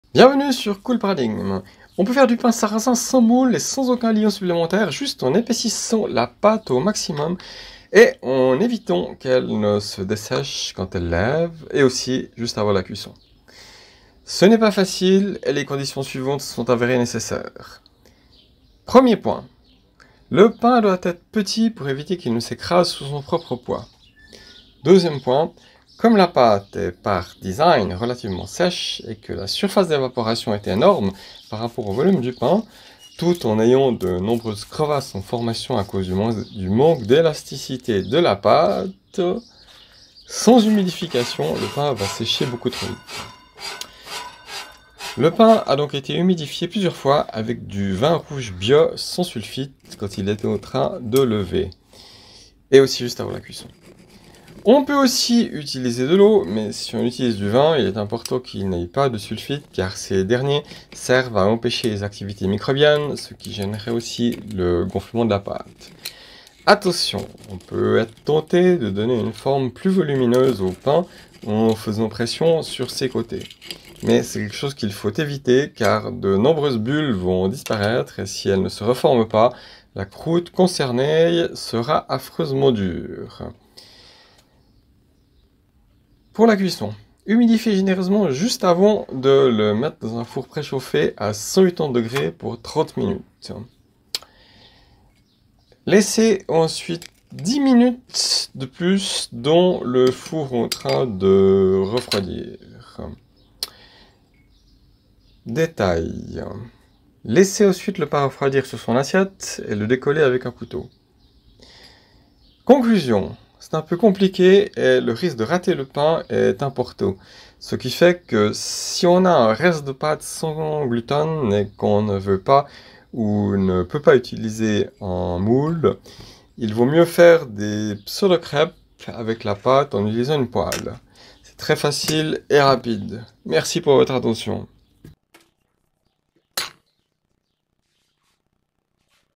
Boulangerie acrobatique : du pain sarrasin sans gluten, sans moule et sans liant !
On peut faire du pain sarrasin sans moule et sans aucun liant supplémentaire juste en épaississant la pâte au maximum et en évitant qu’elle ne se dessèche quand elle lève et à la cuisson.

Ce n’est pas facile et les conditions suivantes se sont avérées nécessaires :

# Premier point : le pain doit être petit pour éviter qu’il ne s’écrase sous son propre poids.

# Deuxième point : comme la pâte est par design relativement sèche et que la surface d’évaporation est énorme par rapport au volume du pain tout en ayant de nombreuses crevasses en formation à cause du manque d’élasticité de la pâte, … ! sans humidification le pain va sécher beaucoup trop vite !
Le pain a donc été humidifié plusieurs fois avec du vin rouge bio sans sulfites quand il était en train de lever ! On peut aussi utiliser de l’eau, mais si on utilise du vin il est important qu’il n’aie pas de sulfites car ces dernier servent à empêcher les activités microbiennes, ce qui gênerait aussi le gonflement de la pâte.

Attention, on peut être tenté de donner une forme plus volumineuse au pain en faisant pression  sur ses côtés, mais c’est quelque chose qu’il faut éviter car de nombreuses bulles vont disparaîtrez et si elles ne se reforment pas , la croûte concernée sera affreusement dure.

Cuisson :
# Humidifier généreusement juste avant de le mettre dans un four préchauffé à 180 degrés pour 30 minutes,
# Laisser ensuite le pain 10 minutes supplémentaire dans le four en train de refroidir.

Détail :
Laisser ensuite le pain refroidir sur son assiette et le décoller avec un couteau.

Conclusion : c’est un peu compliqué et le risque de rater le pain est important ce qui fait que si on a un reste de pâte sans gluten et qu’on ne veut pas ou peu pas utiliser un moule, il vaut mieux faire des pseudo crêpes avec la pâte en utilisant une poêle, c’est très facile et rapide.